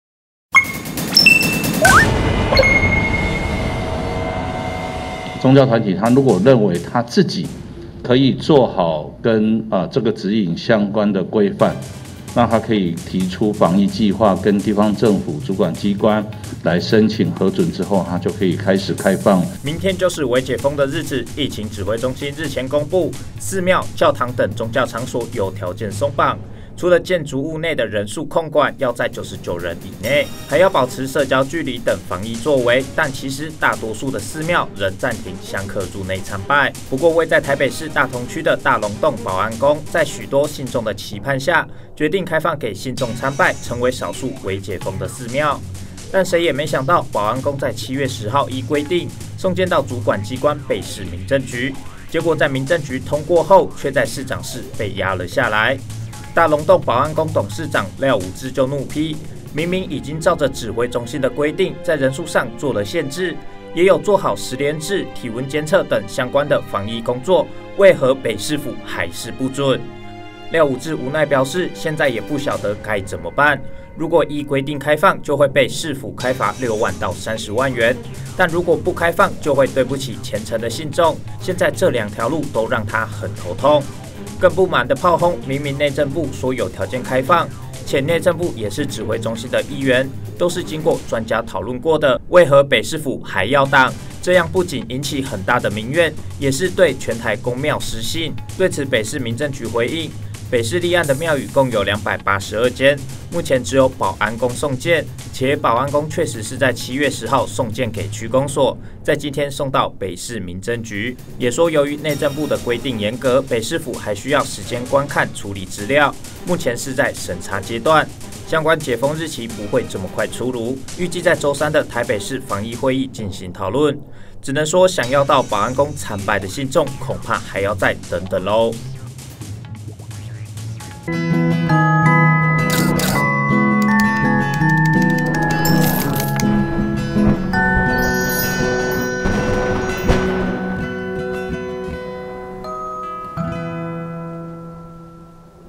台北大龍峒保安宮求解封遭台北市市長柯文哲擋！董座怒批「爛」 嗆：剩2條路可走 獨家 | 台灣新聞 Taiwan 蘋果新聞網
指揮中心宣布7月13日微解封，包含寺廟、教堂等有條件鬆綁，凡進入宗教建物內上限99人，位於台北市大同區的大龍峒保安宮於10日依規送件主關機關台北市政府民政局申請，但保安宮董事長廖武治表示，通過後民政局上報北市府市長室卻「被壓下來」，剛剛民政局局長藍世聰打給他，說是衛生局的意見「說這樣不行」，讓他痛批「真的很爛，就是多一事不如少一事」。對此，民政局回應，今日才收到件，需時間審查。